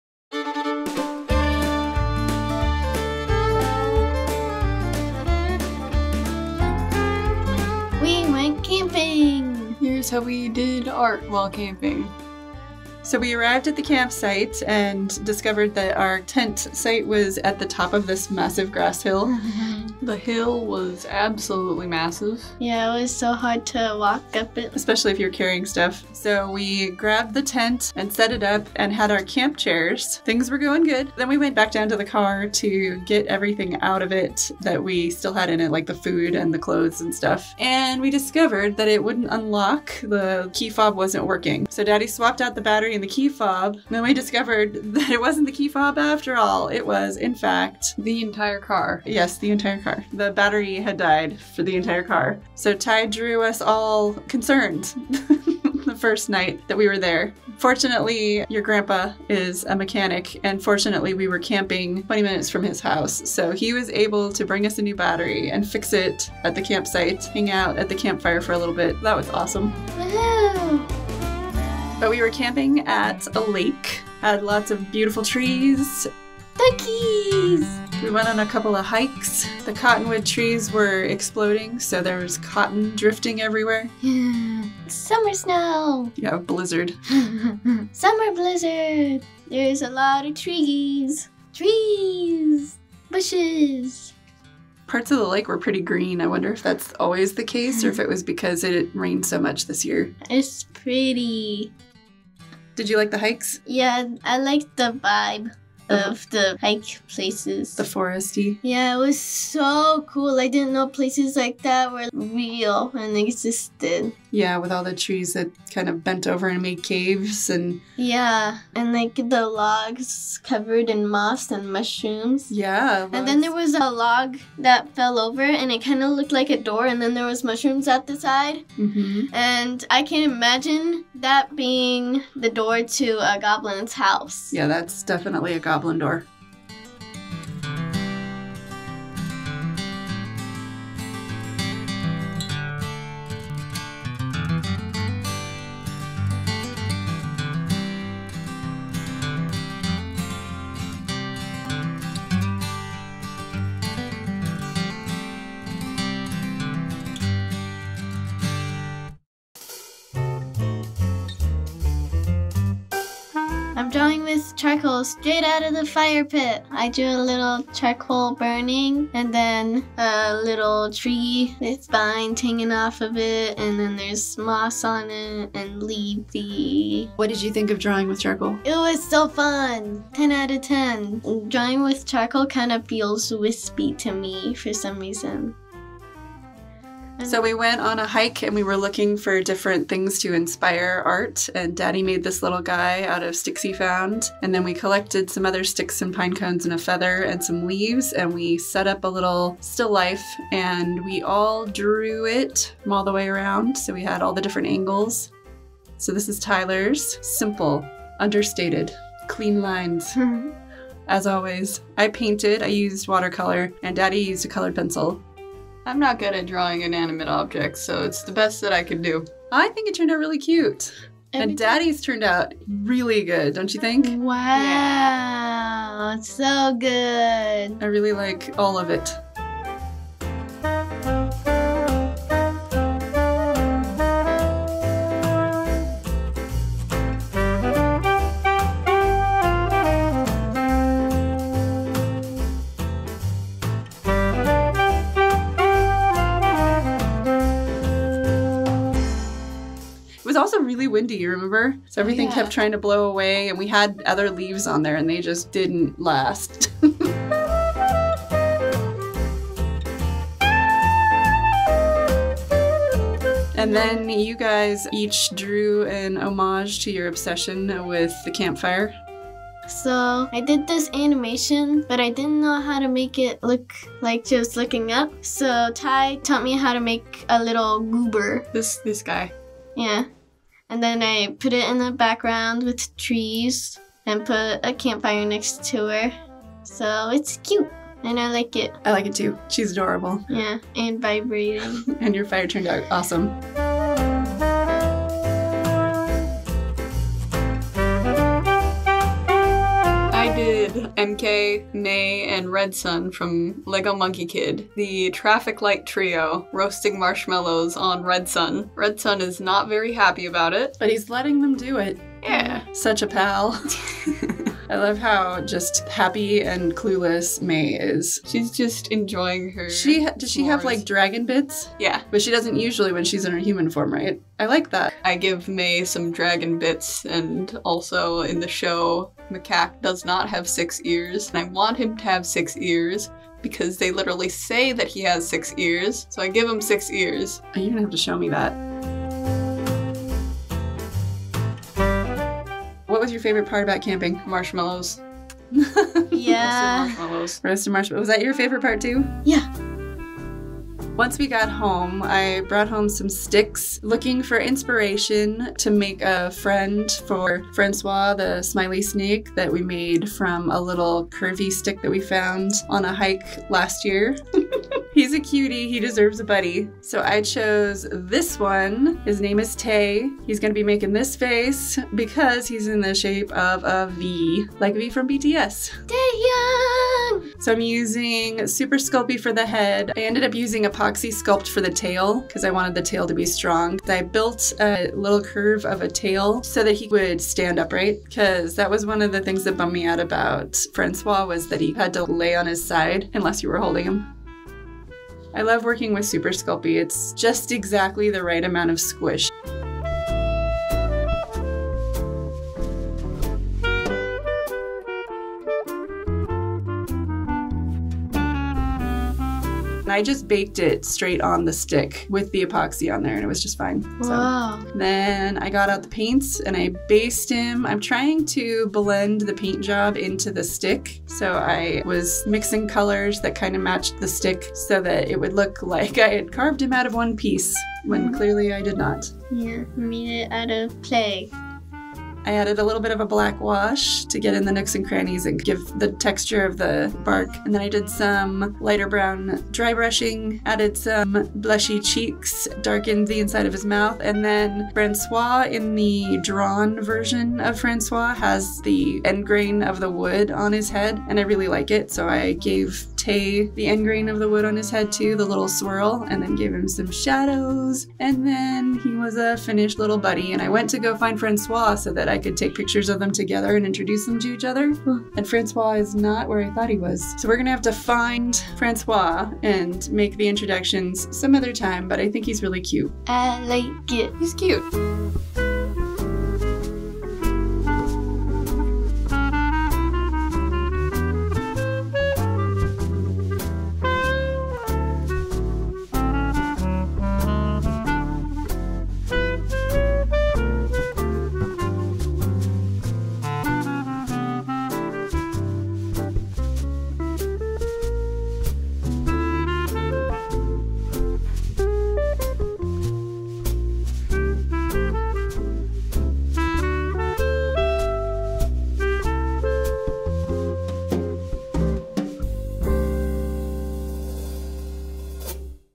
Art Inspired by Our Lovely Summer Camping Trip.
We went on a fun four day family camping trip and did a lot of drawing, hiking and a bit of sculpting.  It was a lovely relaxing time in nature!  ArtVenture!

We love ProductionCrate.com